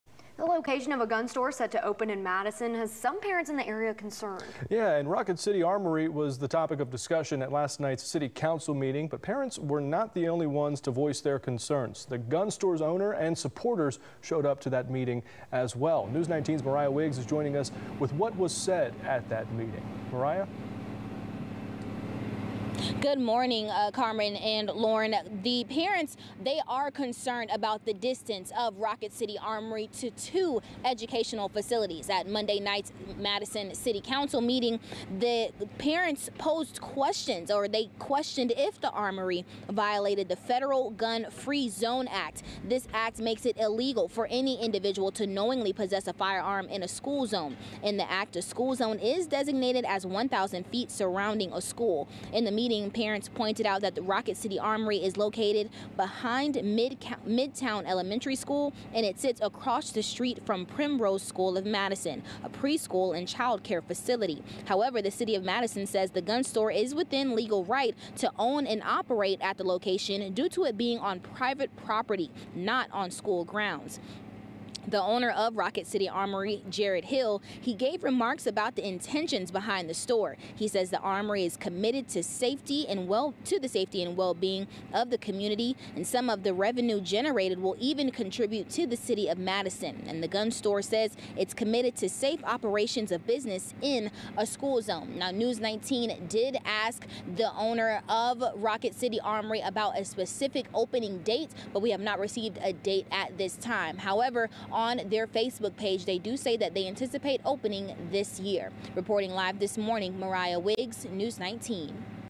Madison Residents Take Concerns About Gun Store Location to City Council Meeting | May 23, 2023 | Ne
Rocket City Armory will soon open less than a thousand feet from Midtown Elementary. This has become a concern for parents with children who are enrolled at the school. Those parents shared their concerns with the Madison City Council.